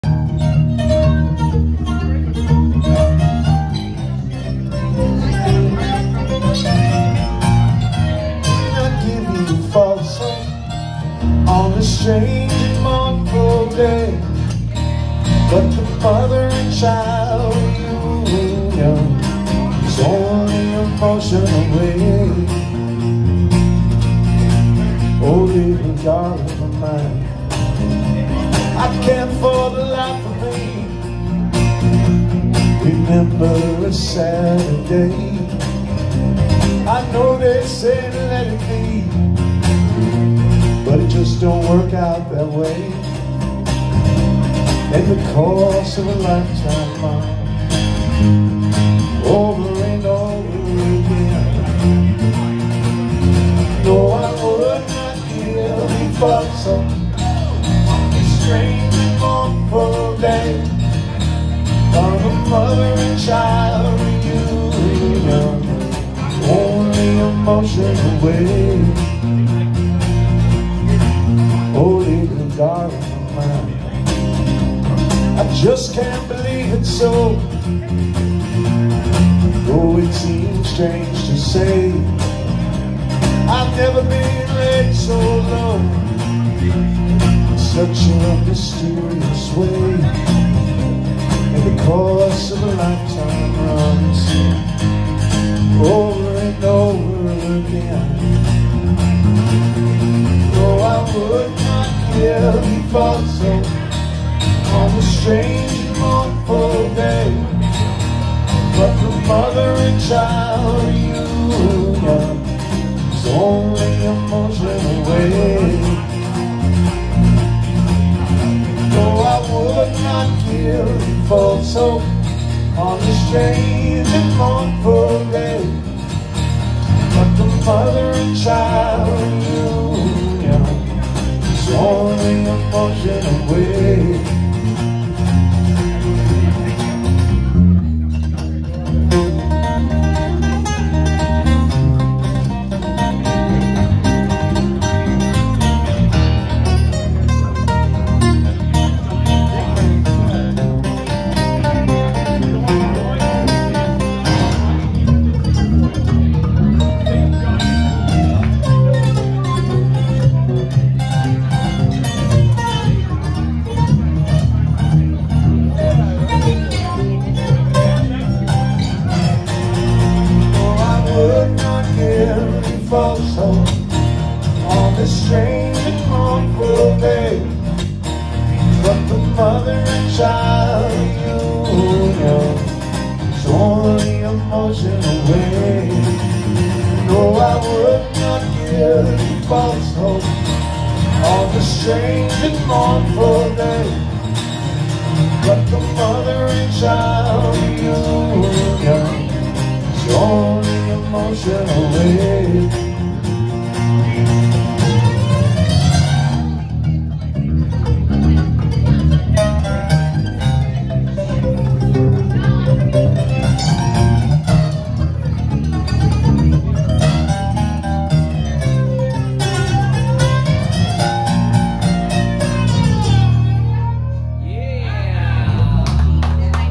Di Leo / Ranno @ Proof 142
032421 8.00 pm. Paul Di Leo & Richie Ranno performing at Proof 142.